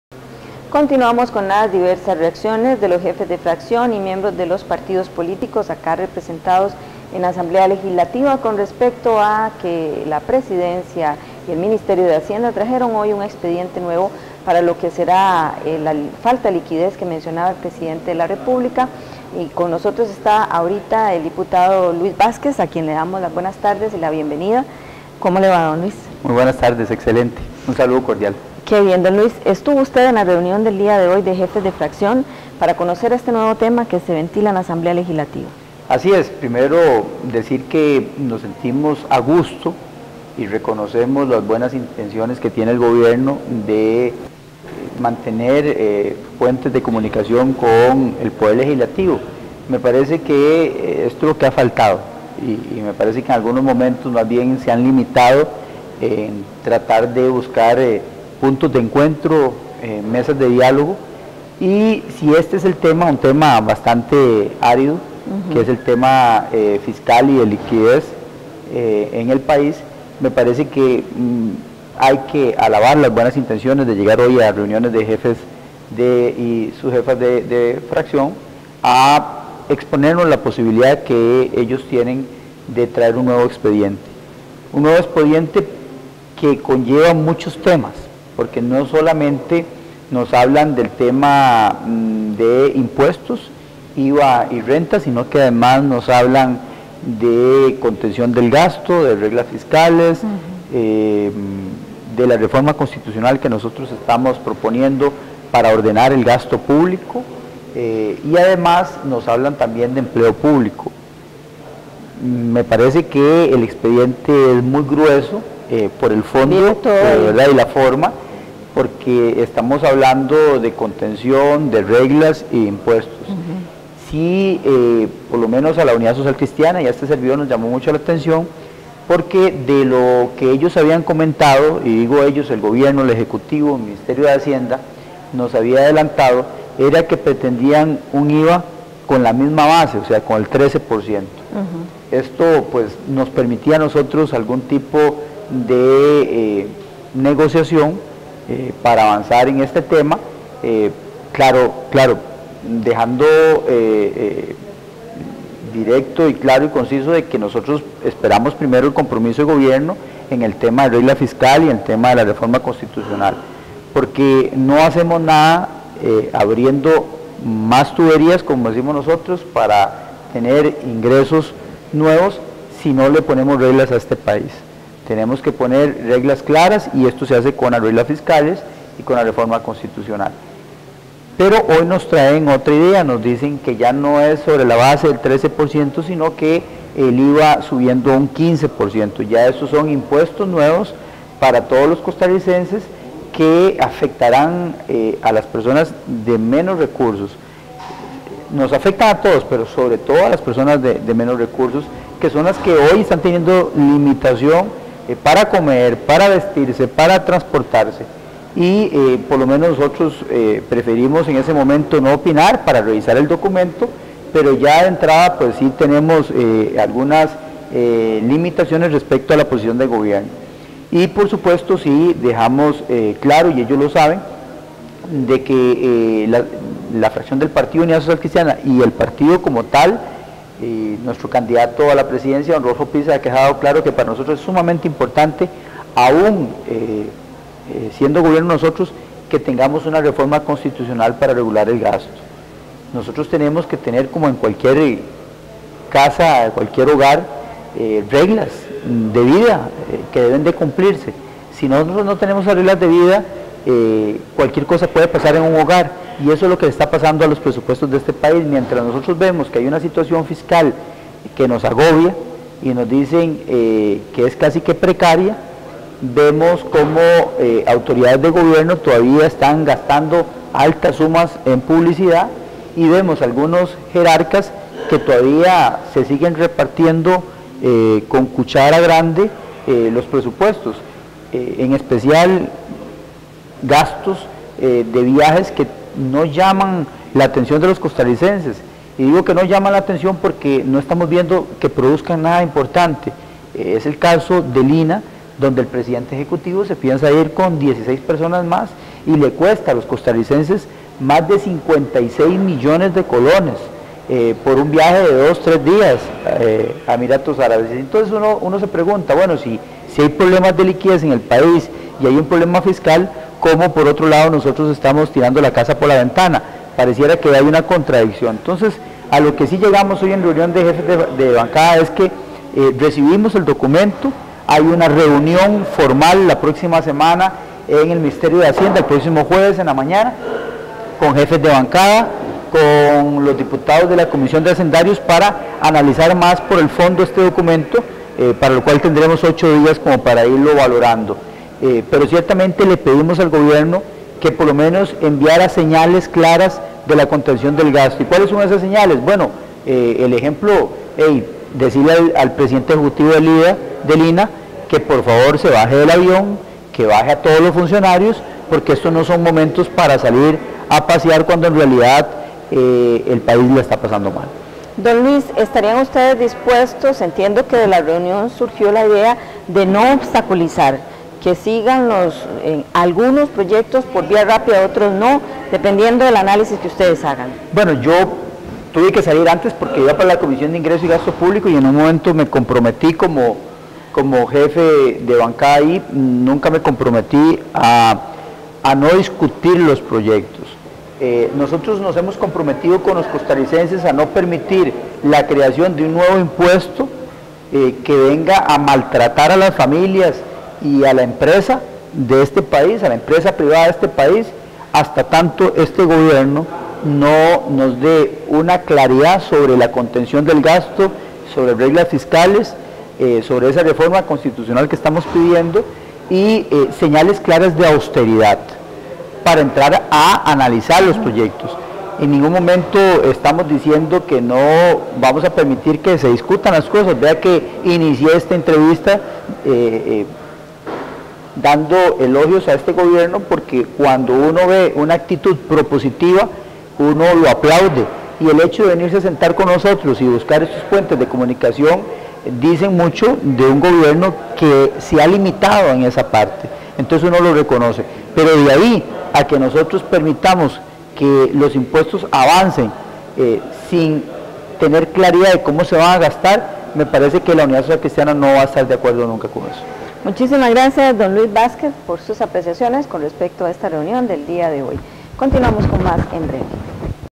POSICIÓN DEL PUSC RESPECTO PROYECTO FORTALECIMIENTO FINANZAS PÚBLICAS
El subjefe de la fracción socialcristiana, Luis Vásquez,  explica a TV Legislativa la posición de su bancada respecto al proyecto de ley de fortalecimiento de las finanzas públicas presentado por el gobierno